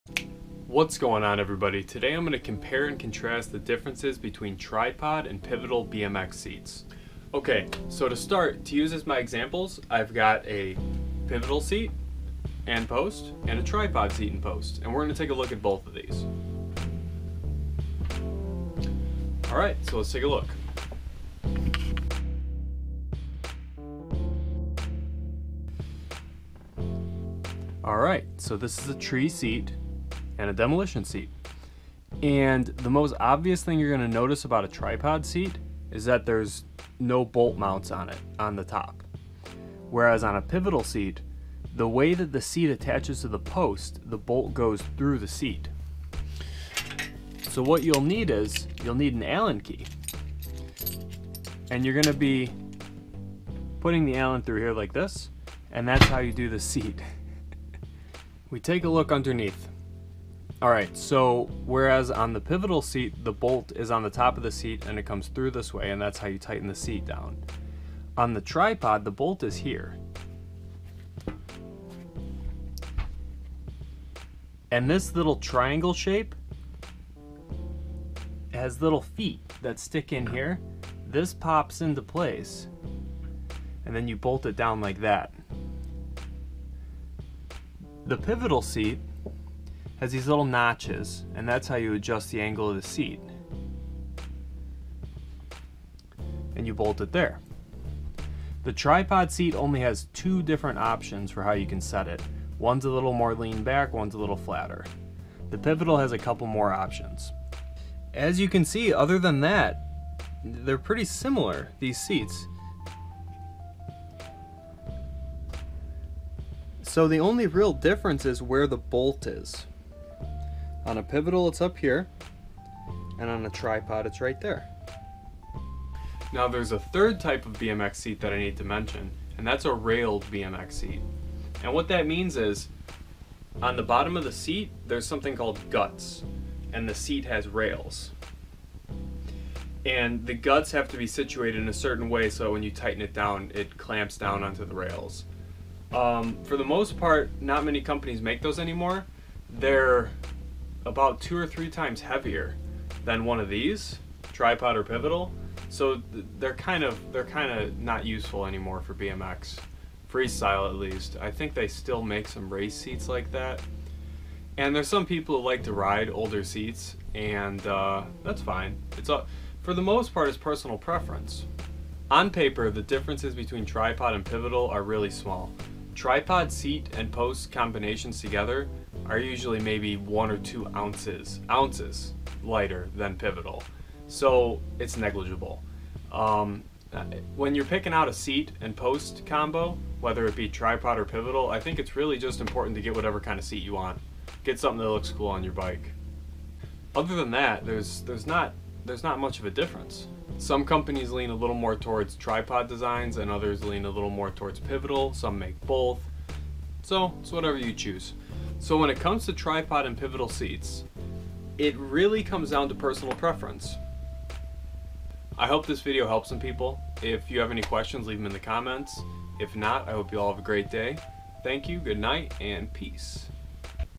Tripod or Pivotal BMX Seat?
In this video, I attempt to compare and contrasts (differences is redundant, I know), Tripod and Pivotal BMX seats. I discuss traditional railed seats as well.  I didn't mention seat/post combos (1 piece), because they are relatively rare nowadays.